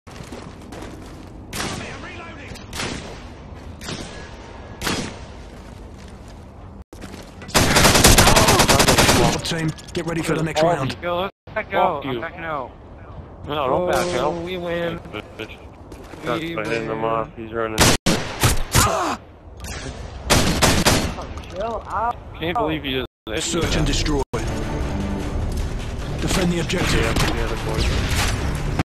Faze Fakie getting slapped by my Wordene 5000
Just a short little clip of the cocky faze fakie getting slapped, Enjoy